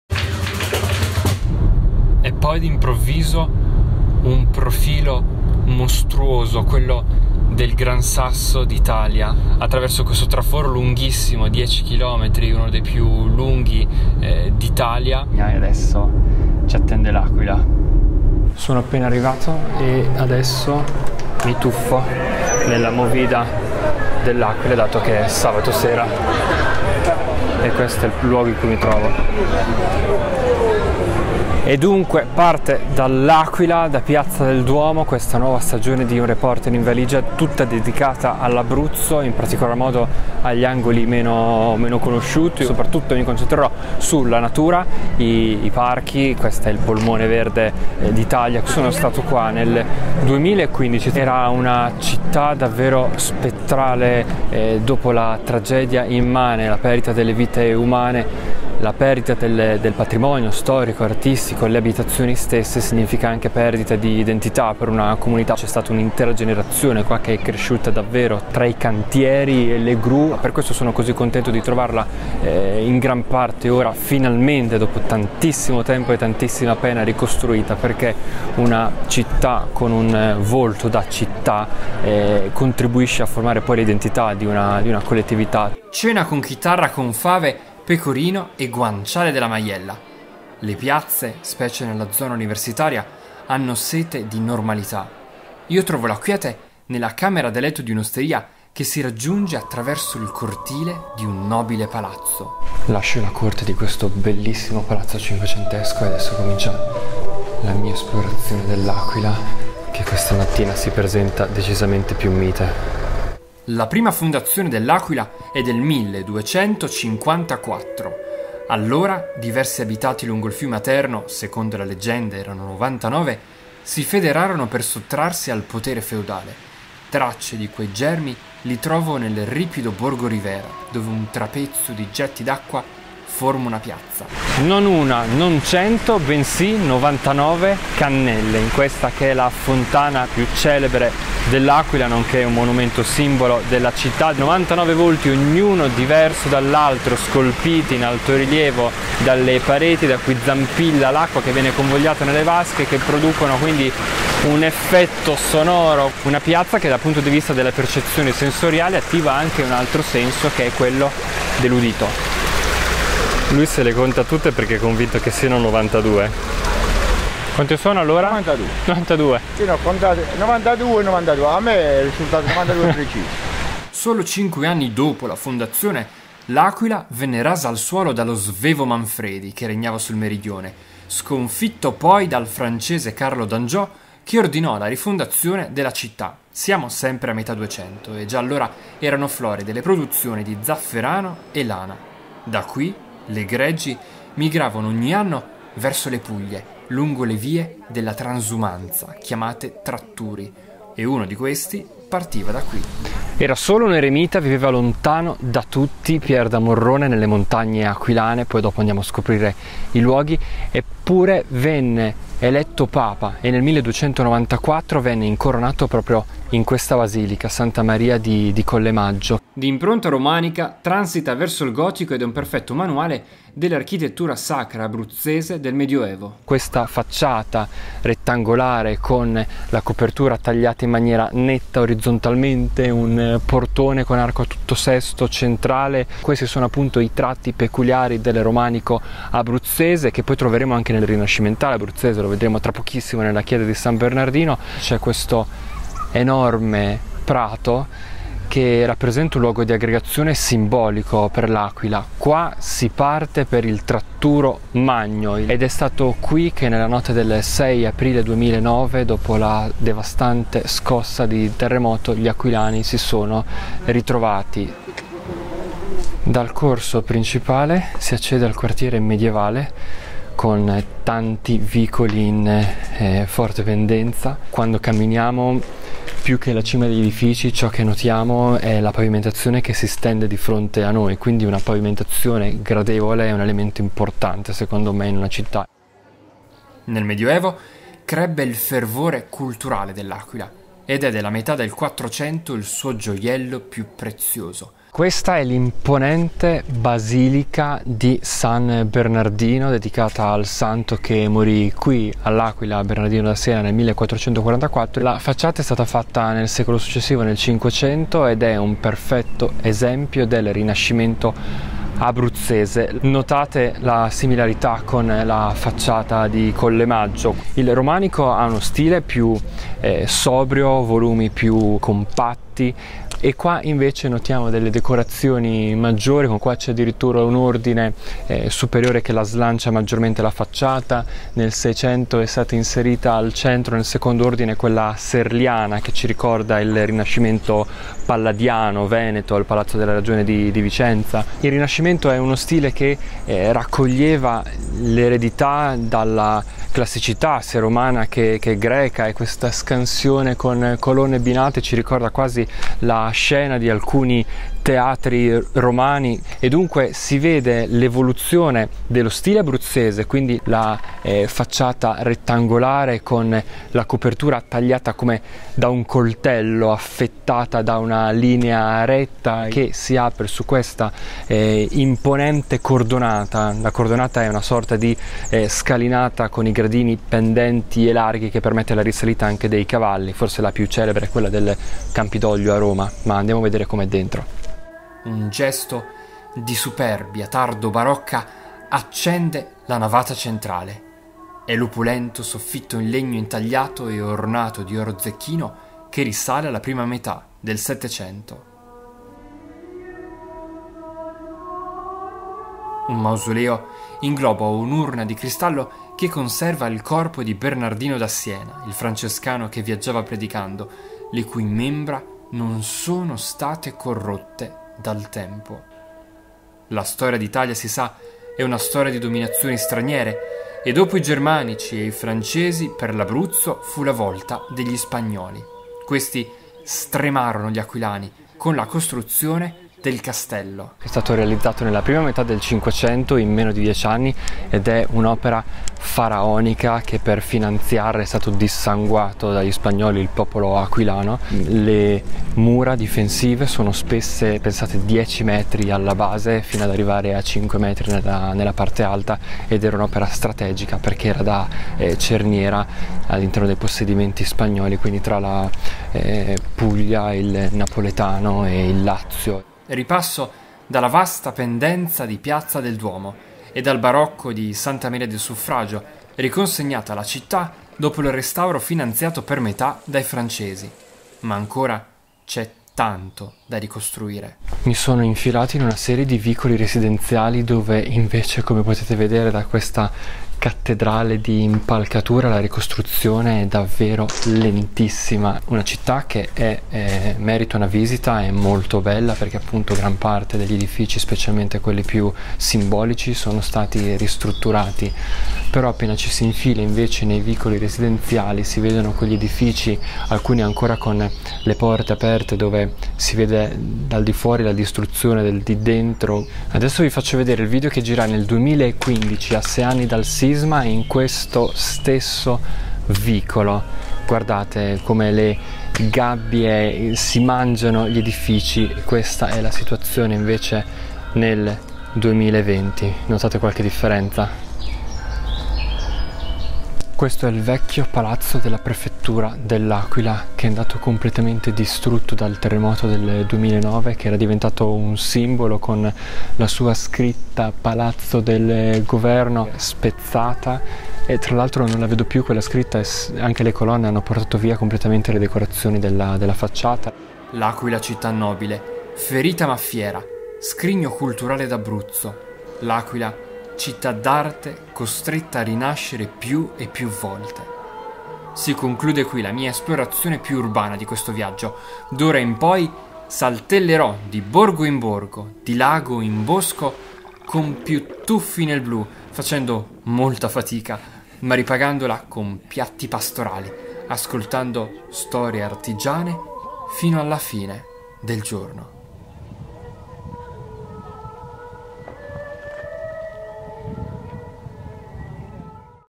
ABRUZZO Ep. 01 - L'Aquila rinata
L’Aquila città nobile. Ferita ma fiera. Scrigno culturale d’Abruzzo. LAquila città d’arte costretta a rinascere più e più volte. La stagione di unreporterinvaligia dal titolo ‘Abruzzo, dove nasce il silenzio’ comincia dallo splendore aquilano che con fatica torna a farsi conoscere dopo il terremoto del 6 aprile 2009. Ero stato nel capoluogo abruzzese nel 2015 quando ancora tra gli edifici scafandrati e le strade senza bar regnava il silenzio. Andate a visitare L’Aquila e a sostenere i suoi tenaci abitanti.